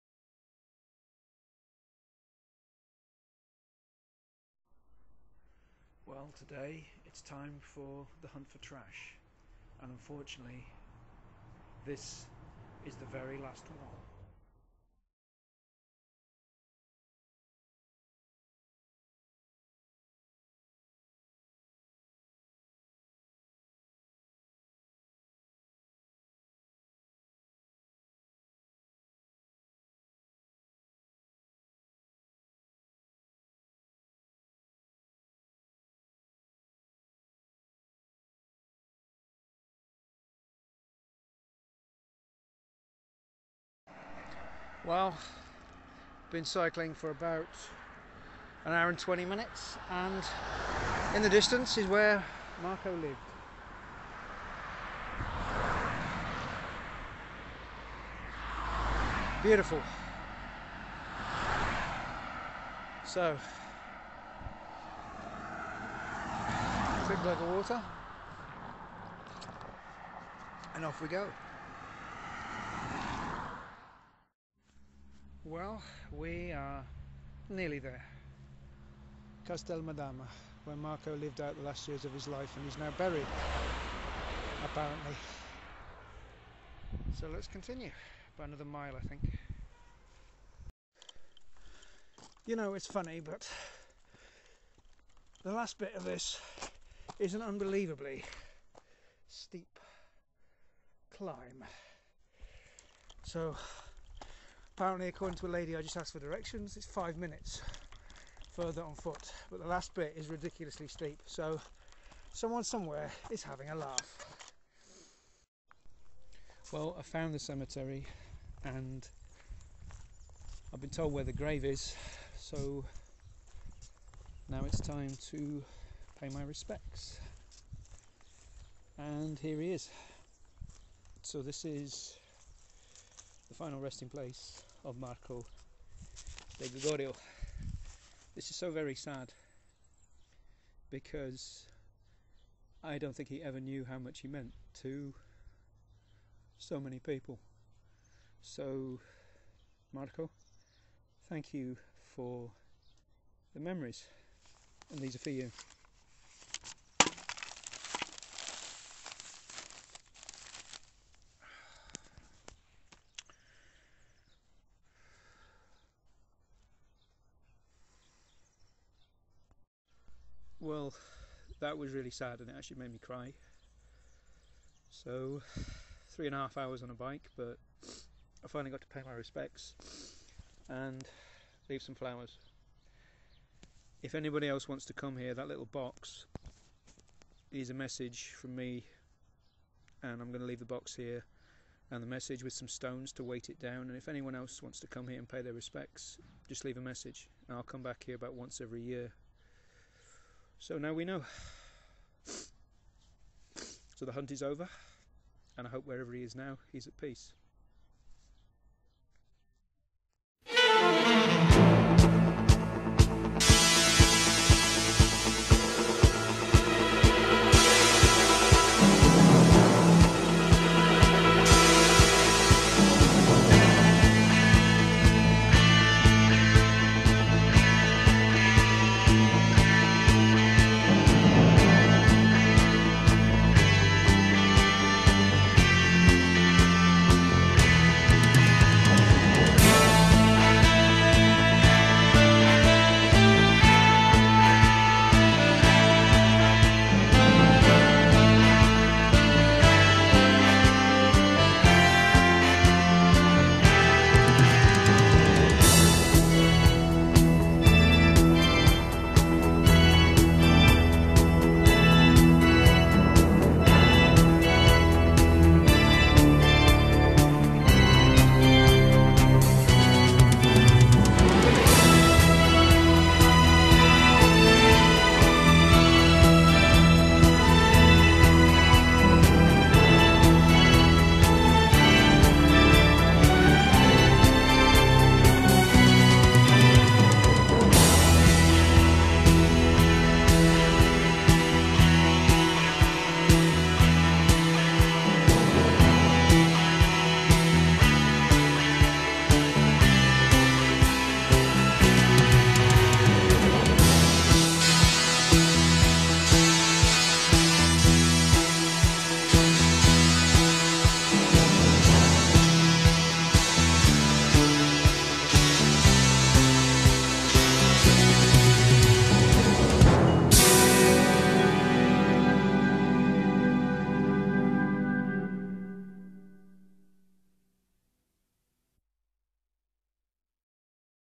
Bronx Warriors: The Hunt for Trash Is Over
On January 31st 2013 actor Mark Gregory (Marco de Gregorio),  who played Trash in Bronx Warriors and its sequel Escape From The Bronx, committed suicide in Castel Madama, Rome.
On March 4th 2022 Roberto Zanni published an article online revealing this.
Up until this point noone knew.
On March 13th 2022 I cycled 26 miles from my house in Rome to the cemetery where Mark is buried.
The hunt for Trash is finally over.